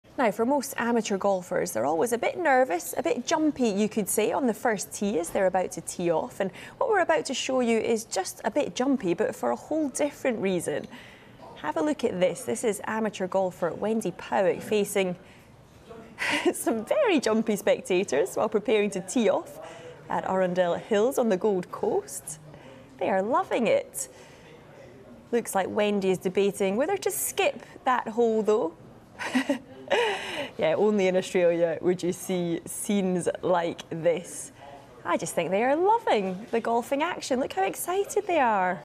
Kangaroos invade golf course in Australia! 🦘
Amateur Australian golfer Wendy Powick faced an unusual hazard while teeing off at Arundel Hills on the Gold Coast.